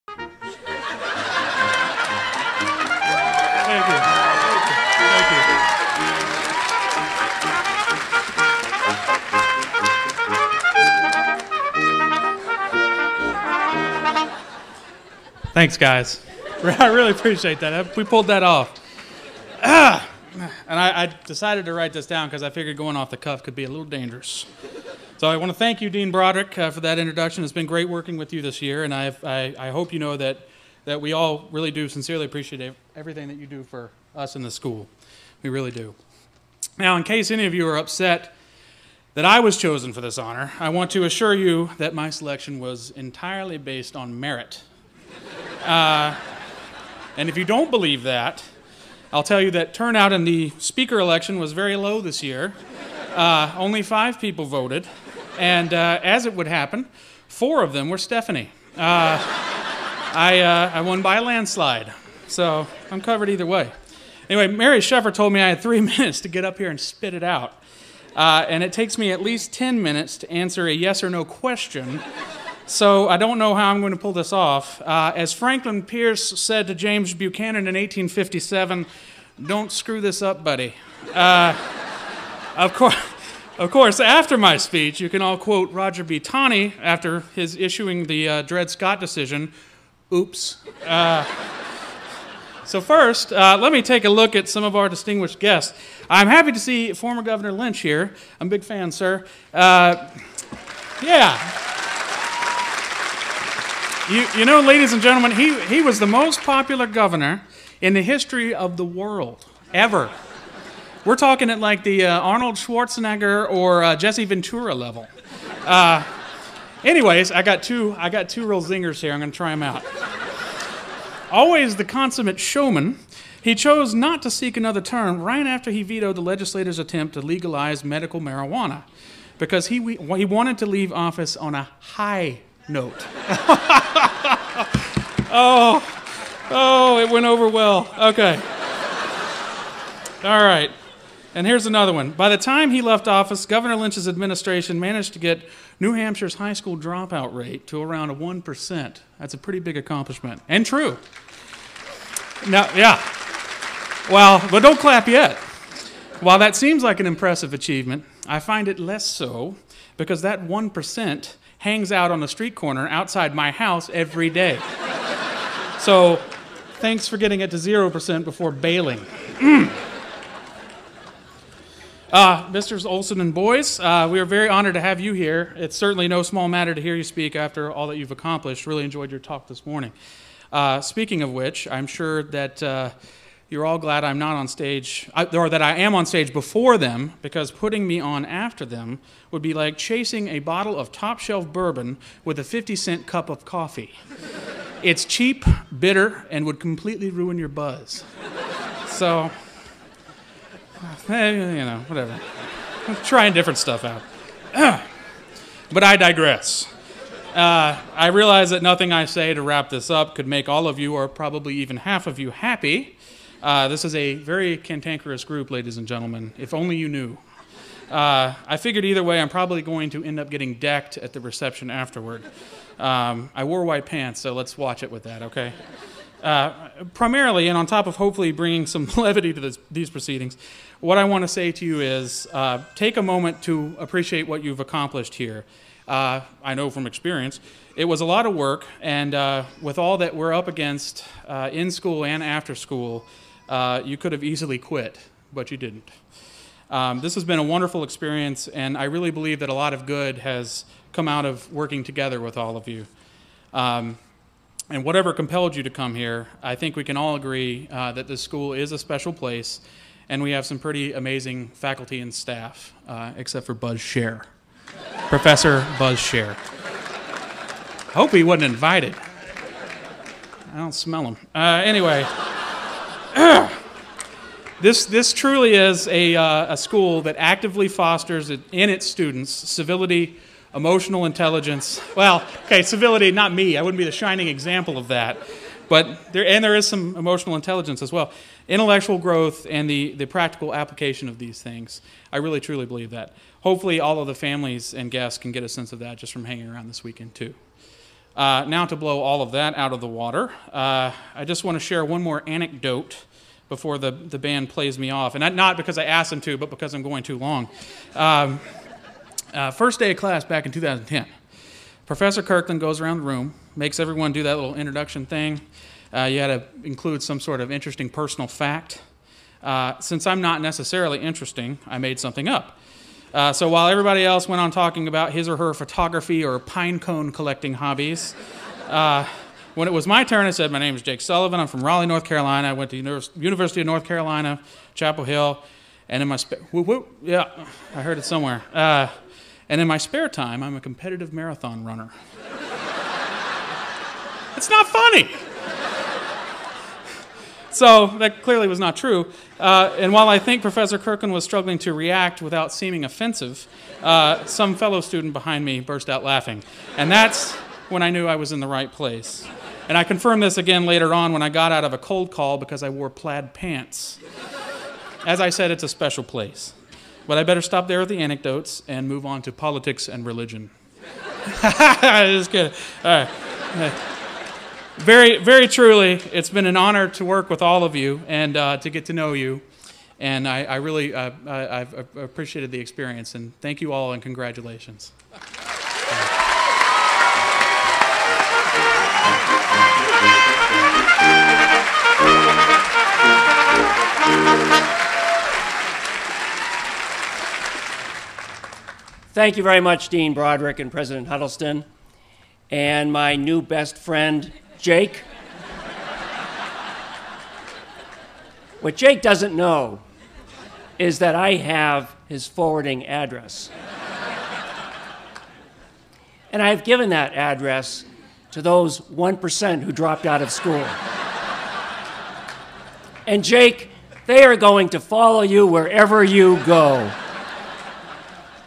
Commencement '13: Jake Sullivan
Jake Sullivan JD 13 makes his commencement address